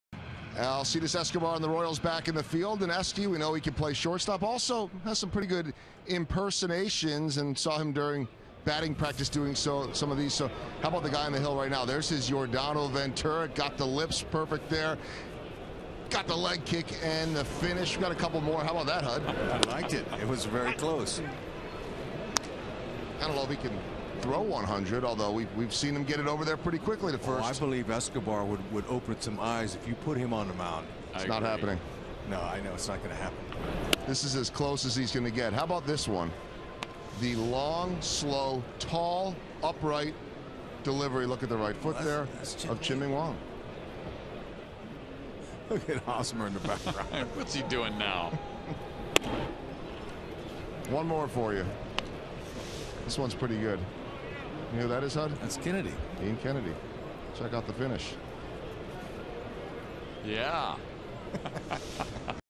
KC@TB: Escobar has fun with impressions
Alcides Escobar does impressions of Yordano Ventura, Chien-Ming Wang and Ian Kennedy during batting practice before a game against the Rays